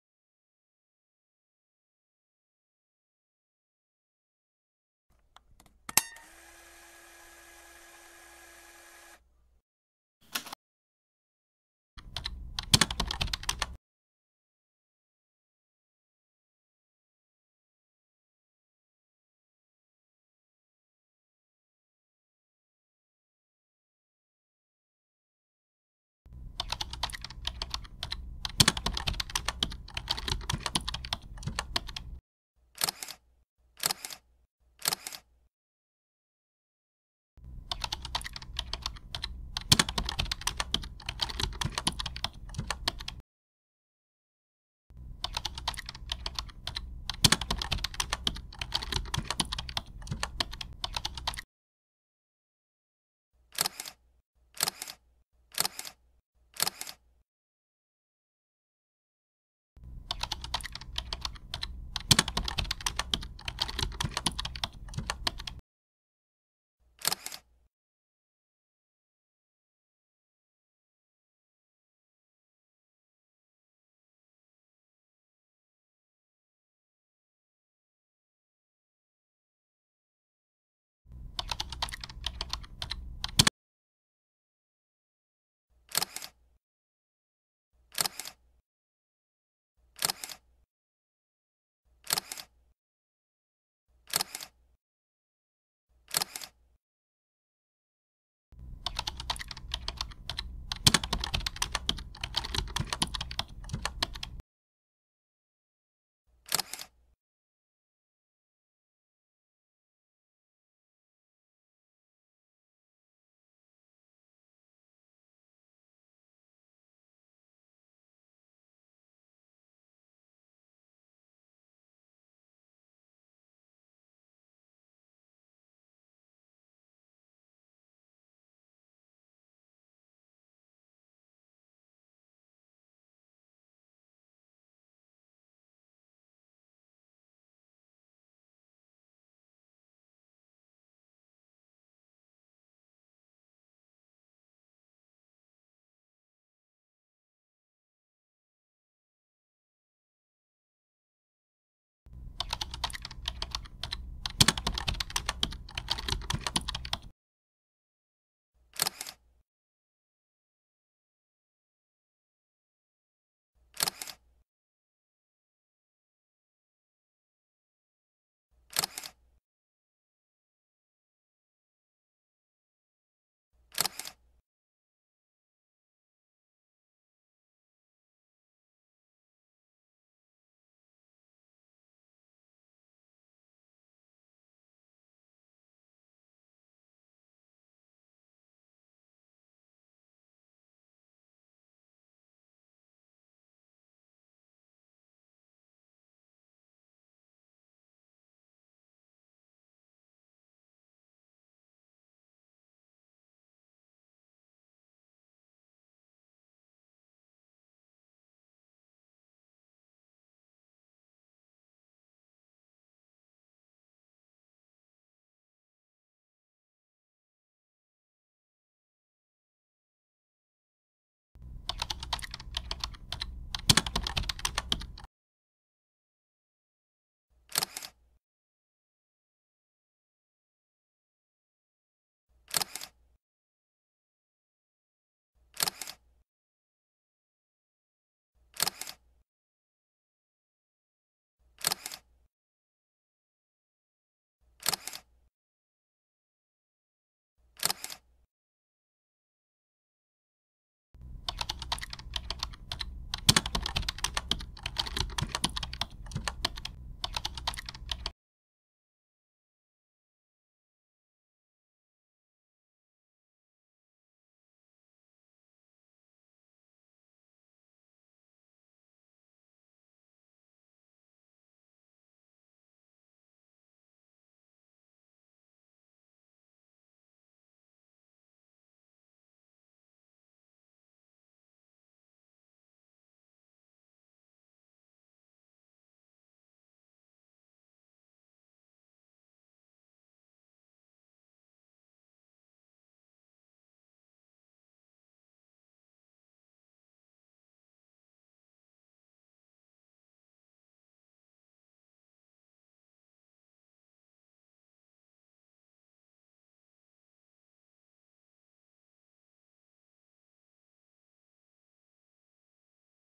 Brain Craft 2022 I Muang-Ake Smart District, "ReVerse"
Waste is regarded as one of the most serious issues in Muang-Ake, a small community with a large population due to the abundance of accommodations such as education, shops, residential, and sporting facilities.